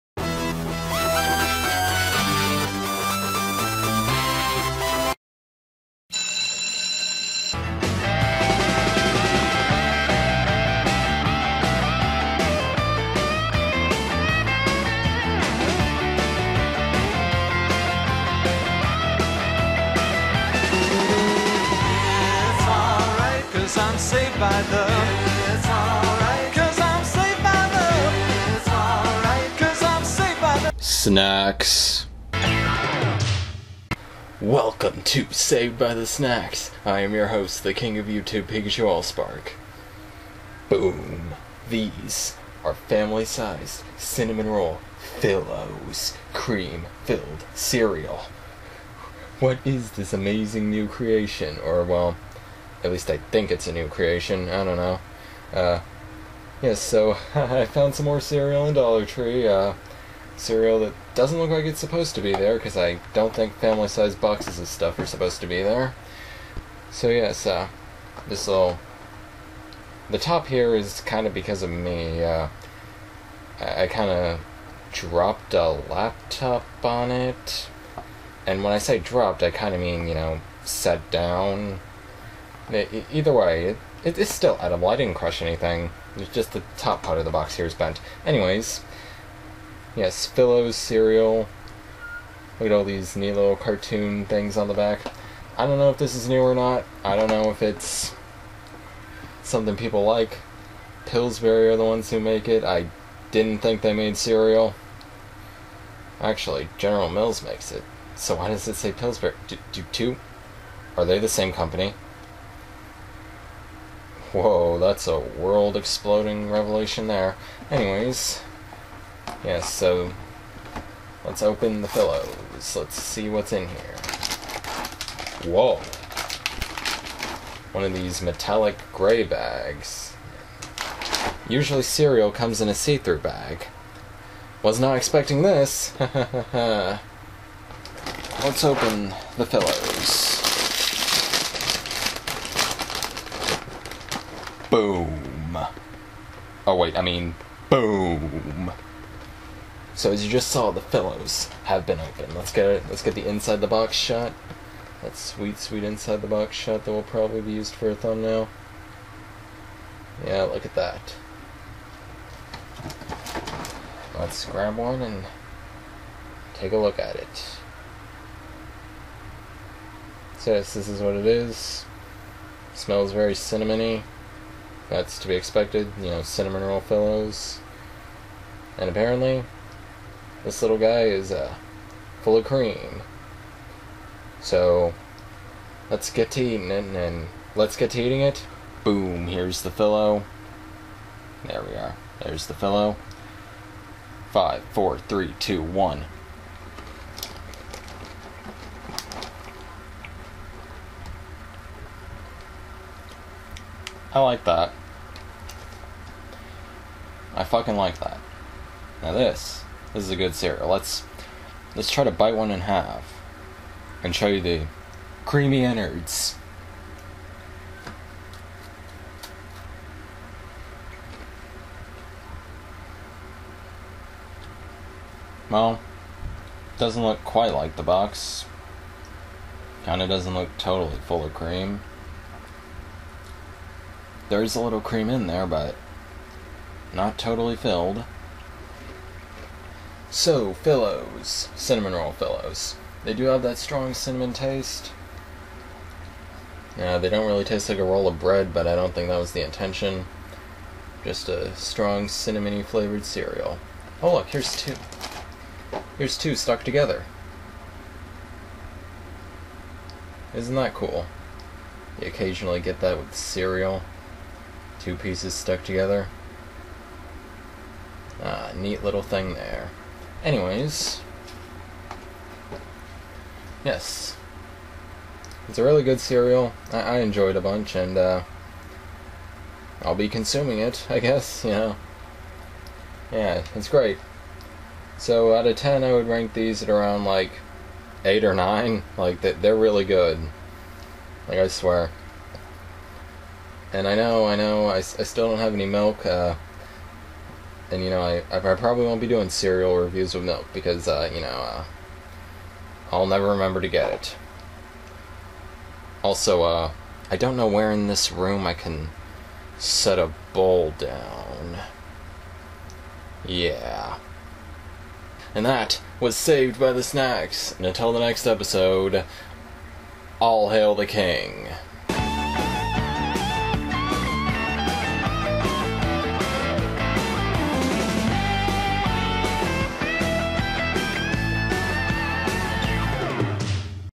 The Unboxing Hour: Cinnamon Roll Fillows Cereal
You ever eat cereal and think, WHY IS THERE NO CREAM IN THIS?! Well, if that's what your average cereal eating session is like, oh boy do I have the cereal for you!